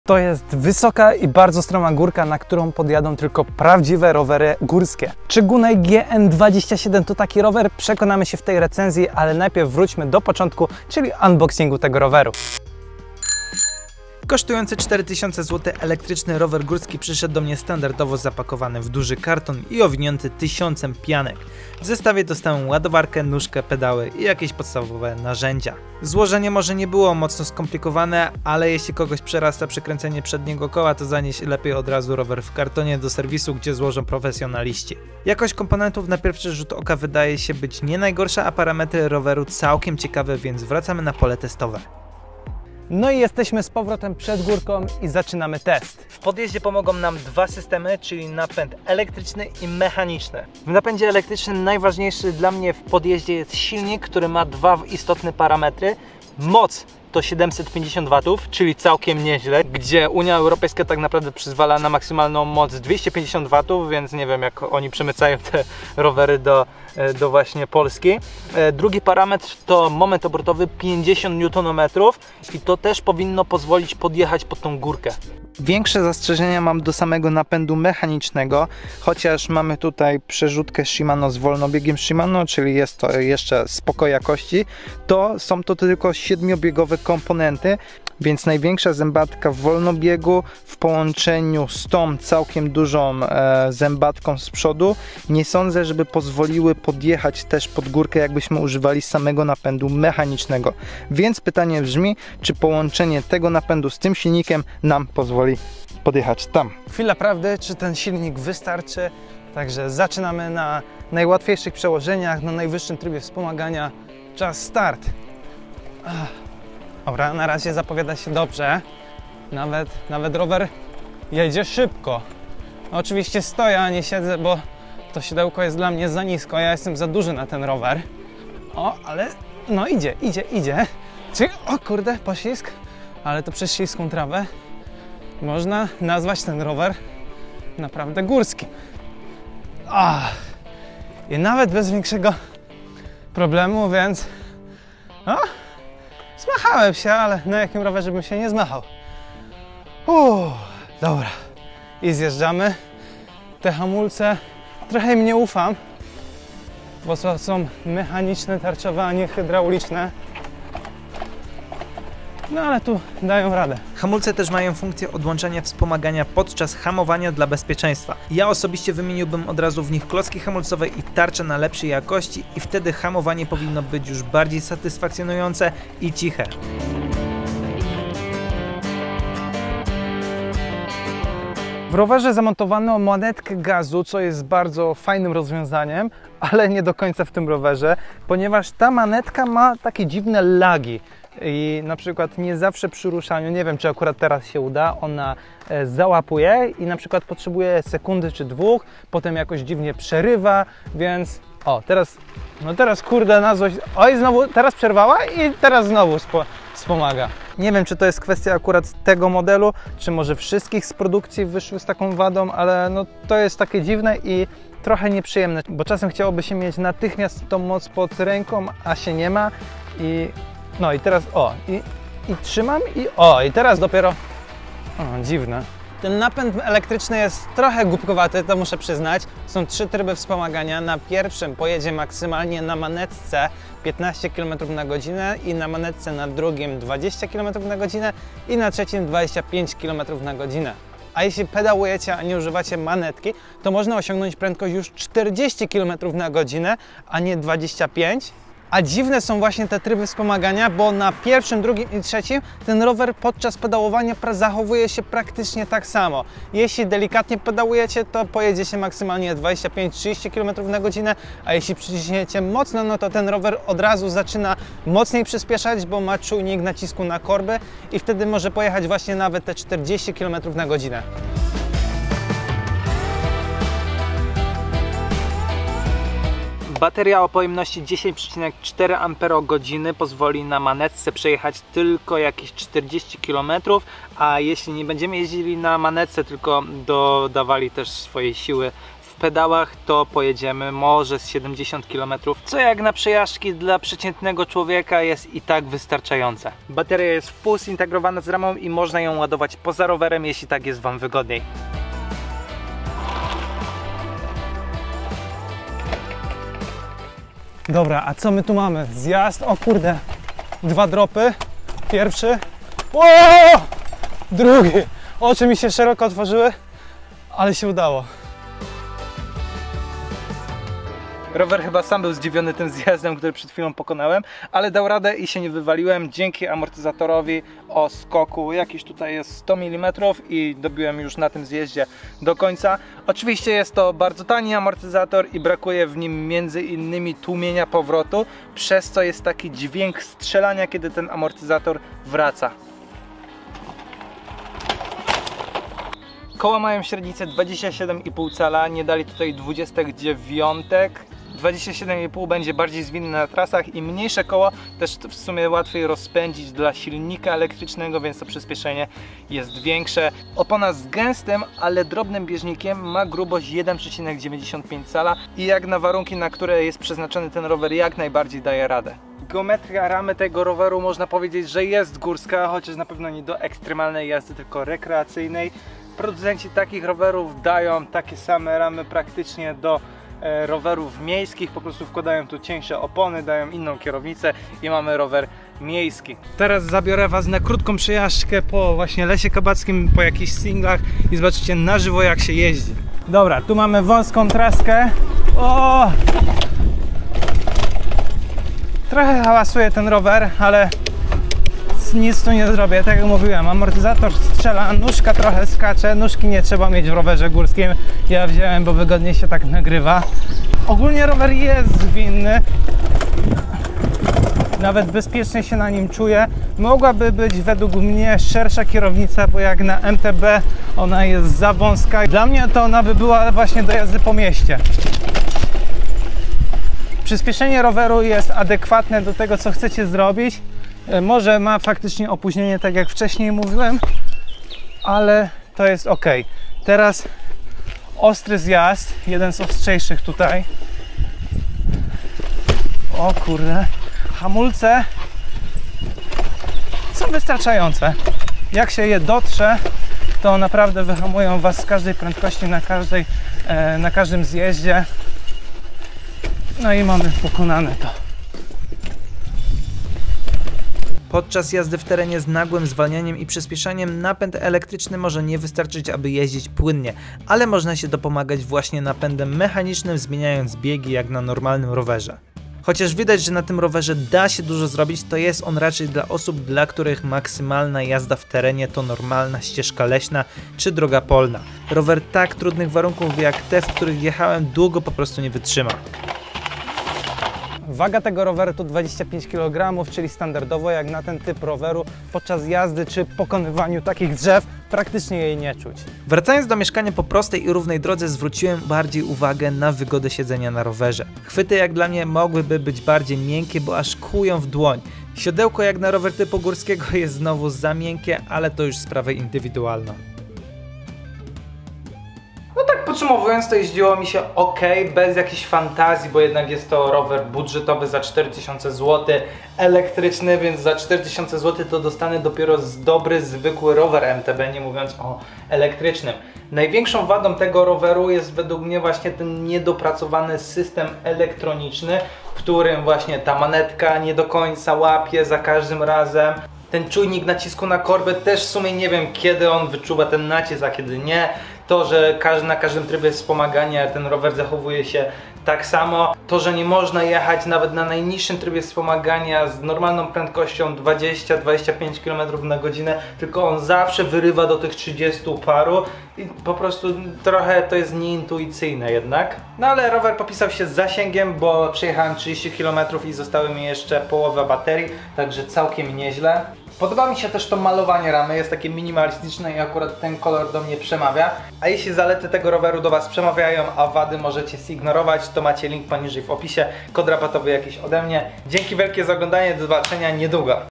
ELEKTRYK MOCNY W TERENIE? - GUNAI GN27
Recenzja roweru elektrycznego z predyspozycjami do jazdy w terenie. Czy faktycznie się do tego nadaje czy może to tylko pozory. Sprawdźcie czy Gunai GN27 jest dla Was.

LINK DO AMAZONA